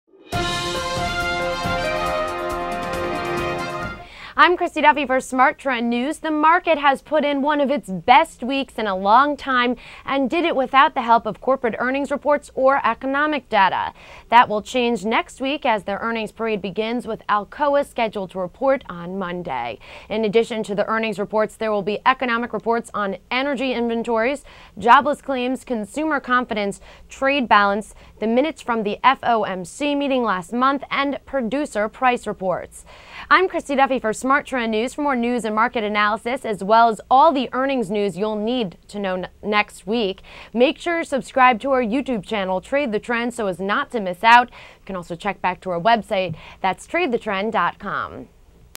News Update: Market has a good week despite a lack of news
The market put in one of its best weeks in a long time and did it without the help of corporate earnings reports or economic data. That will change next week as the earnings parade begins with Alcoa (AA) scheduled to report on Monday. In addition to the earnings reports there will be economic reports on energy inventories, jobless claims, consumer confidence, trade balance, the minutes from the FOMC meeting last month, and producer price reports.